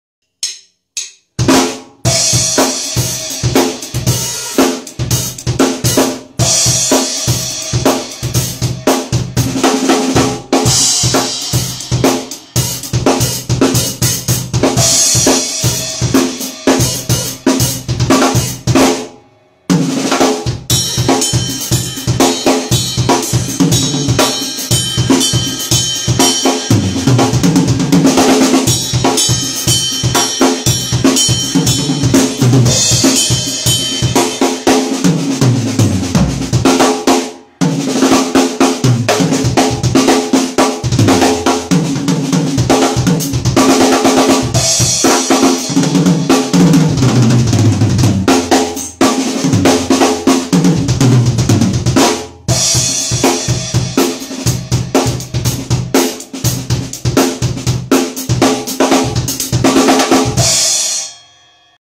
Sound of PEARL ALL BIRCH SHELL
Bas drum 22x16
tom 1 - 10"
tom 2 - 12"
tom 3 - 13"
flor tom 16"
Snare drum Pearl free loating 3,5" coper.